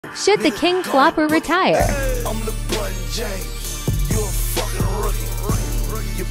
🏀 😂 👑 🐐 Should the NBA LA Lakers The King Flopper Lebron James retire? Lose again to Nuggets!
🏀😂👑🐐 The debate rages on! Should NBA legend and LA Lakers' very own "The King," LeBron James, consider retirement? Join the discussion as we dissect his recent performance and the Lakers' matchup against the Nuggets.

🔥 In this video, we take a closer look at LeBron's career and recent games, sparking conversations about his future in the league. Will he retire with a legacy unmatched, or does he have more to prove?

👍 Whether you're a die-hard fan or just curious about the ongoing debate, hit that "Like" button, share your thoughts in the comments, and don't forget to subscribe to stay updated on all things NBA. Let's keep the conversation going! 🏆🏀